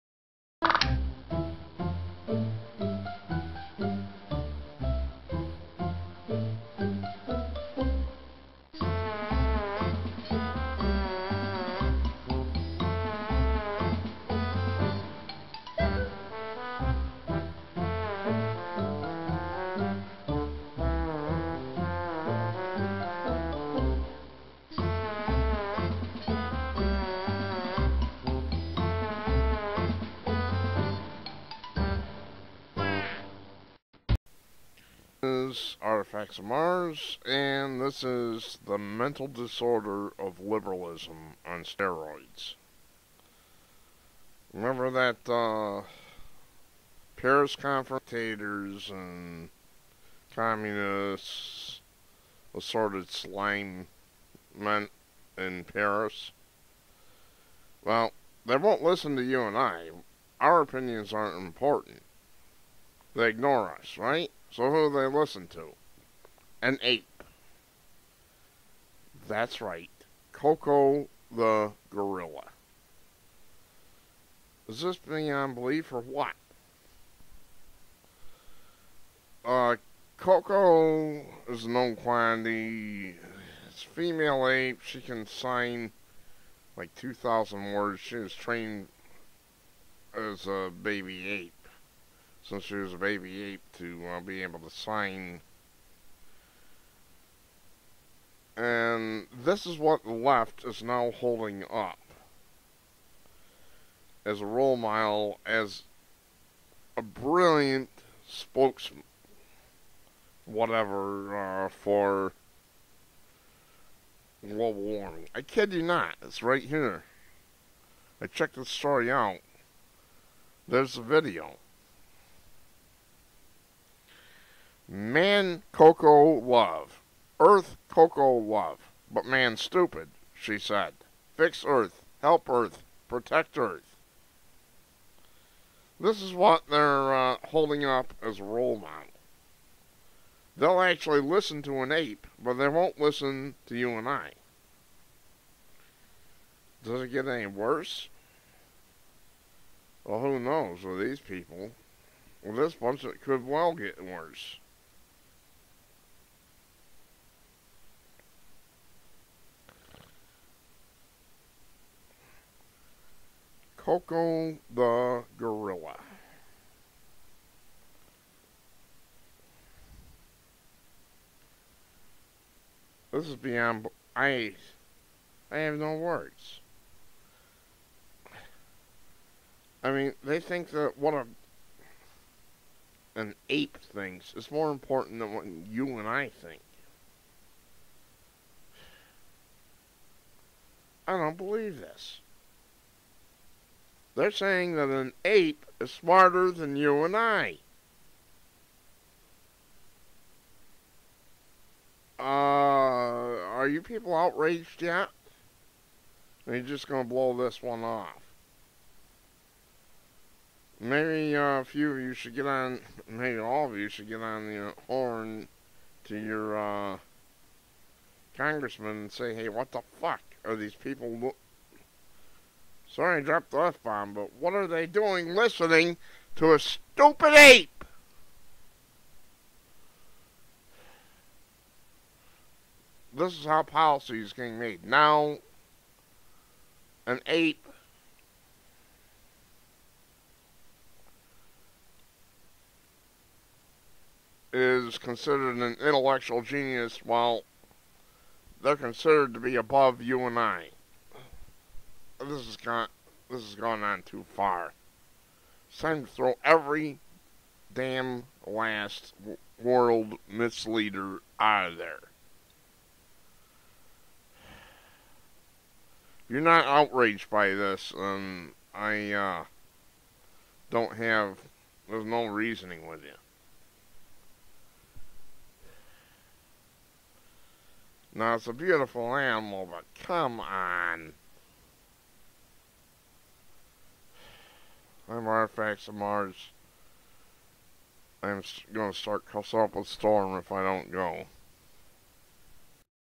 World misleaders would rather listen to an ape than you and I!
Koko the gorilla is now being held up by the Left as an intellectual genius. The ape said in sign language "Man stupid" and "fix Earth".
  Even worse is the fact that the world misleaders who met in Paris to screw us over actually watched a video of this brilliant creature signing her message.